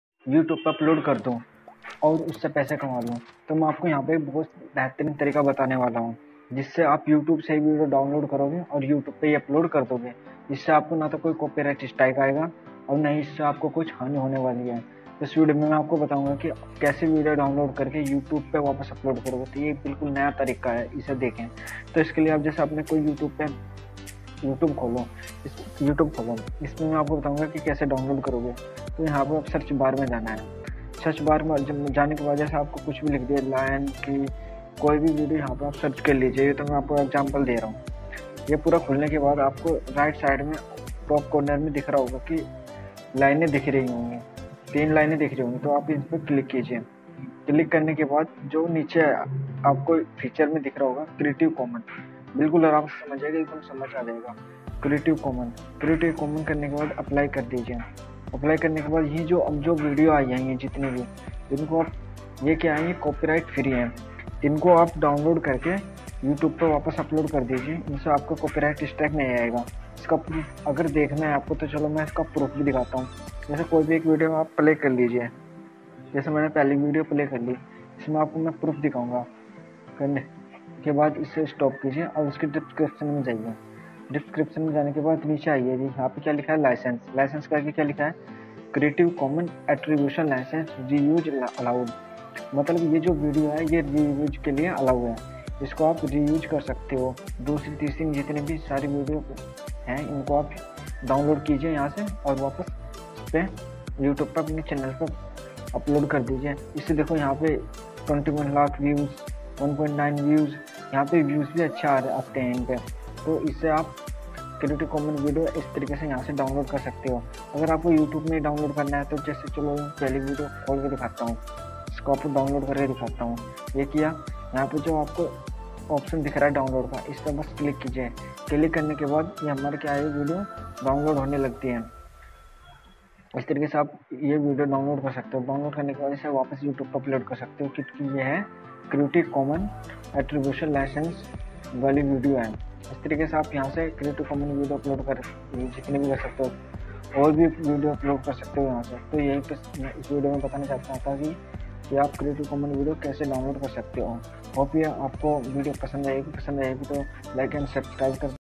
How to download Video From YouTube | How to Download Creative Common Videos | By My Update 1M
How to download Video From YouTube | How to Download Creative Common Videos | Copy Paste Work

Es video me btaya gya ki aap YouTube se video download karke apne YouTube channel per kaise upload kar sakte ho
Aapko iss video me btaya gya hai aap bina Video bnaye ya shoot kiya dusro ki video ko upload kar sakte ho
Ye video puri dekhe aapki sab samaj me aa jayega
How to download creative CommonVideos from YouTube and upload on your YouTube channel
Dosto aap creative Common Videos YouTube se download karke bahoot sara paisa kma sakte ho
Toh video pura dekhe

Thanks by
My Update 1M